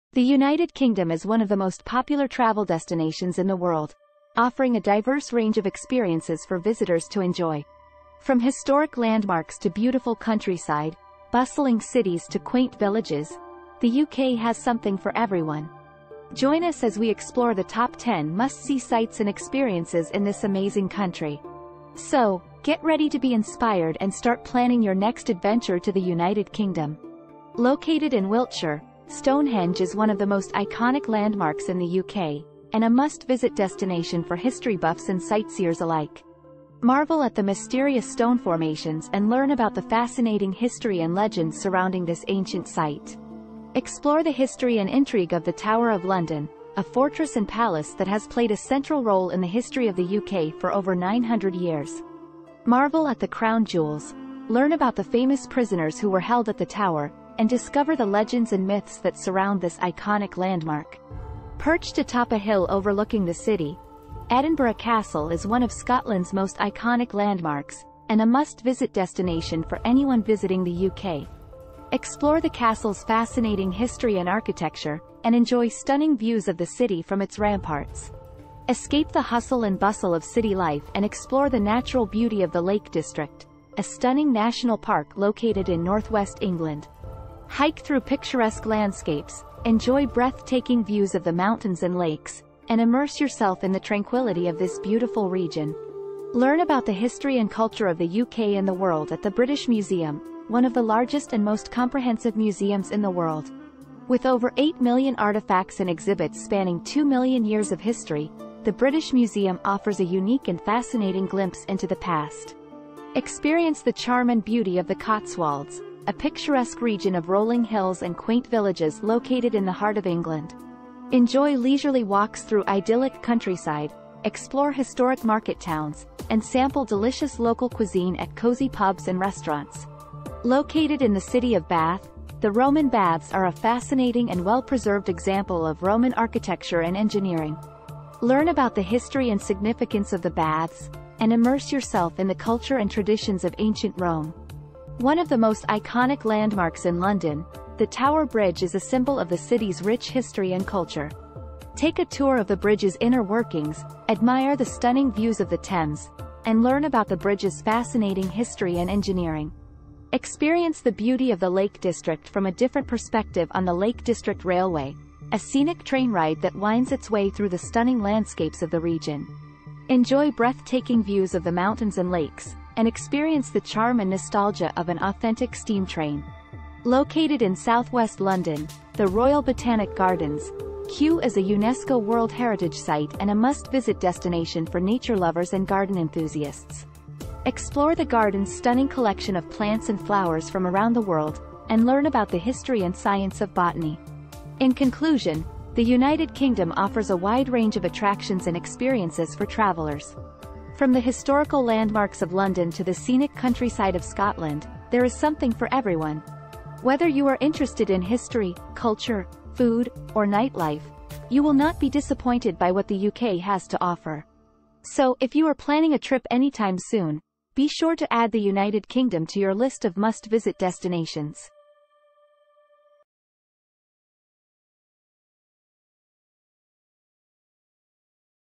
Top 10 Things To Do In United Kingdom!
In this video, you will figure out what are some of the top 10 things to do and visit in United Kingdom.